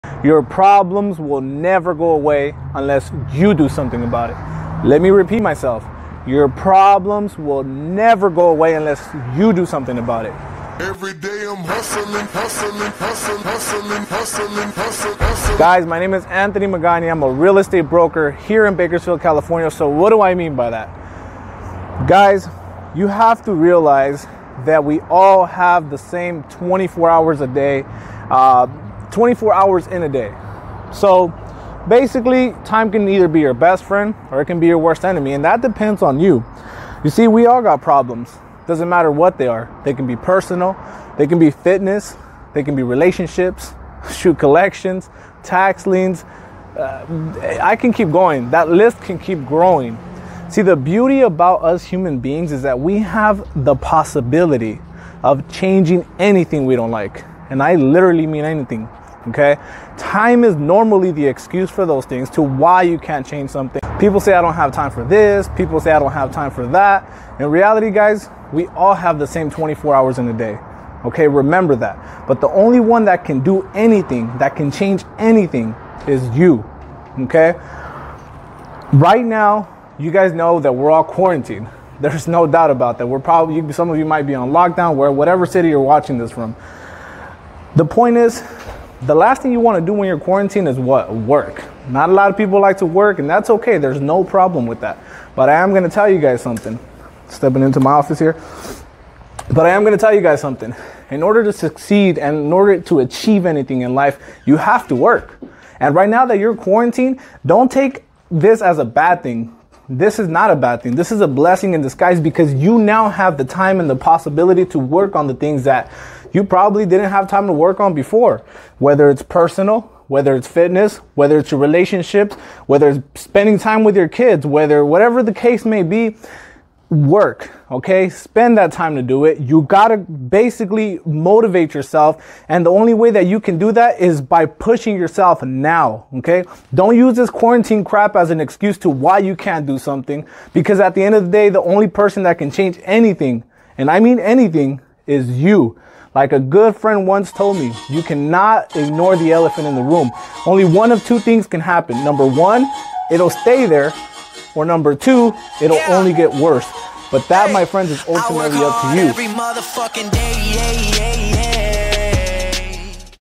Life doesn't happen to you, it happens for you 🧘‍♂️🤯
I'm tired of hearing people complain about not being able to do things because they're "quarantined". It's moments like these that'll truly determine how bad you really want something in life!